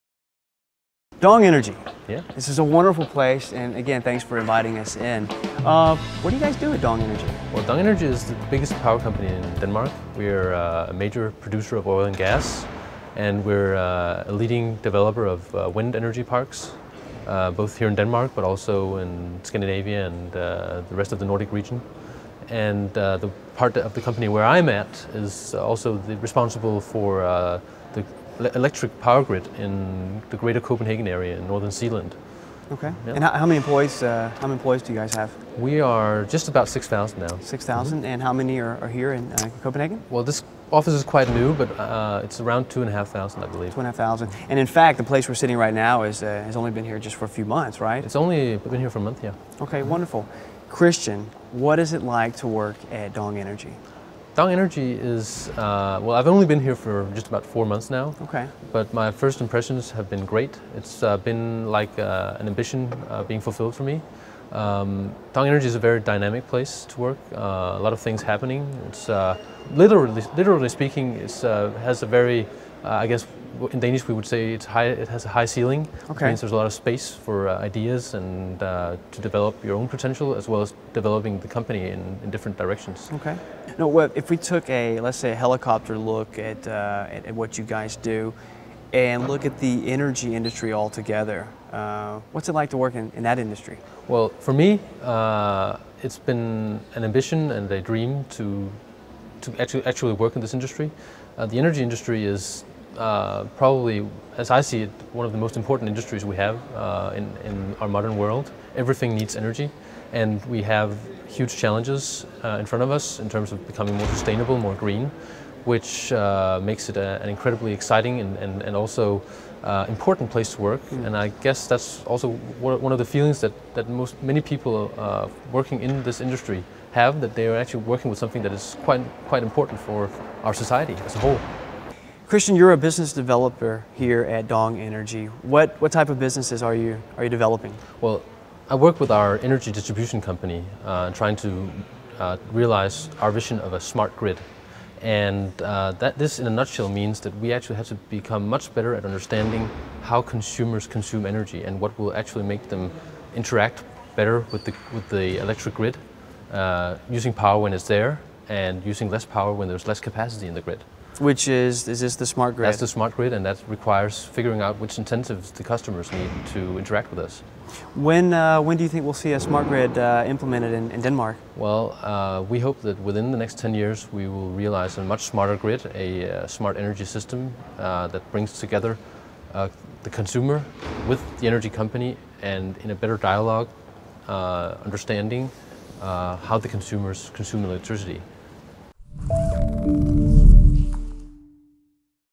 Welcome To My World: DONG Energy: What is DONG Energy and what do you do?
Interview with Christian Eika Frøkiær at DONG Energy